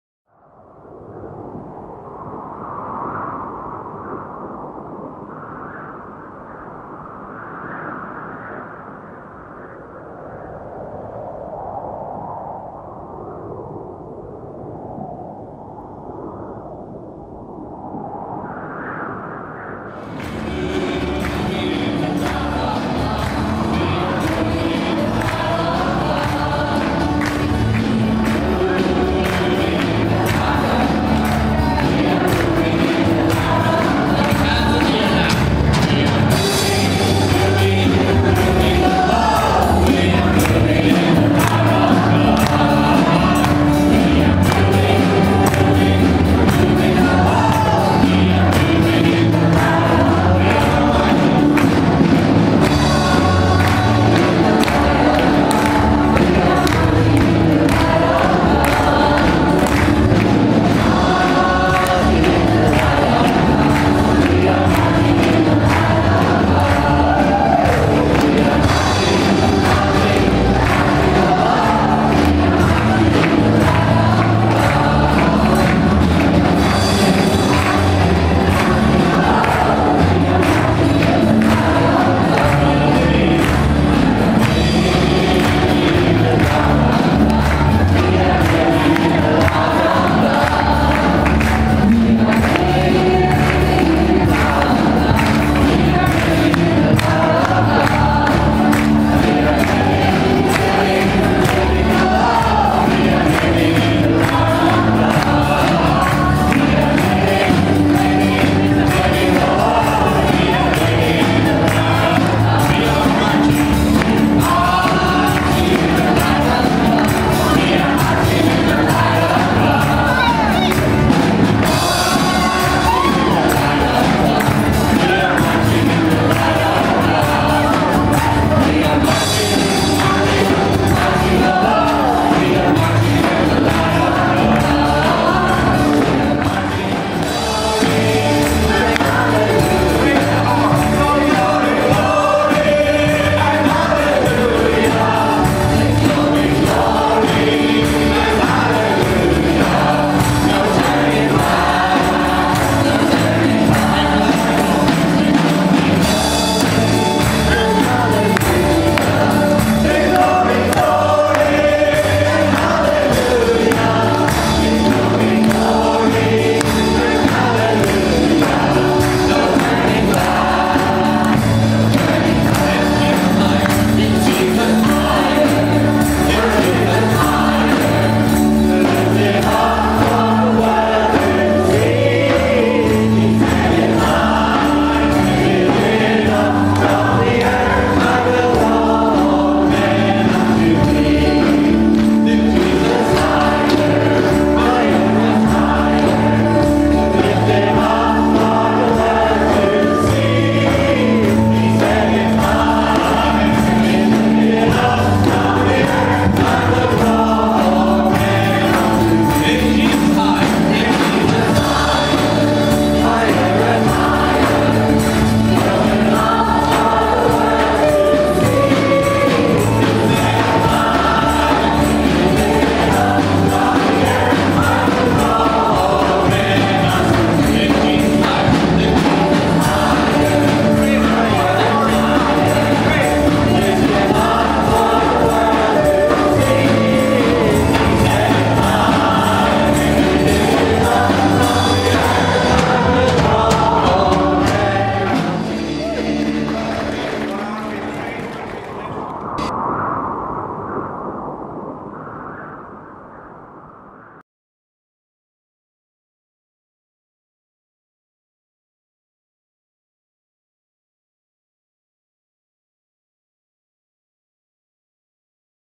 Celebrating Pentecost in Dublin 2012.
A Day of Charismatic renewal organised for Pentecost  by the Irish Charismatic Renewal Service Committee.